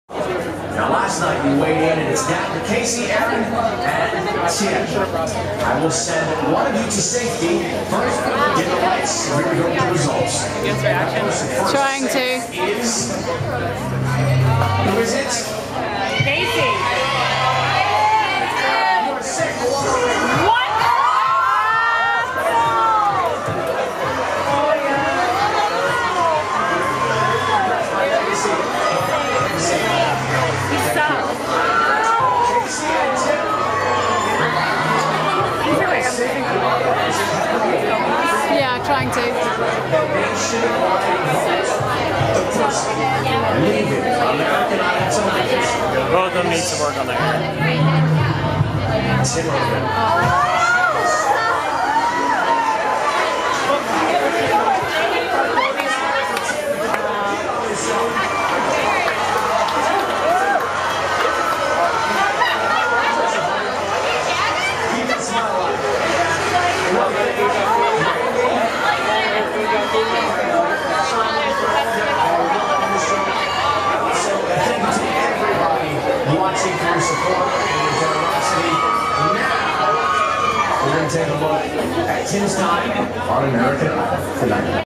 Kris Allen - Idol Gives Back - Hard Rock Cafe, DC
Kris Allen's reaction to the results on American Idol during his appearance at the "Idol Gives Back" watch party at the Hard Rock Cafe in Washington, DC on April 21, 2010.  Apologies for the poor lighting.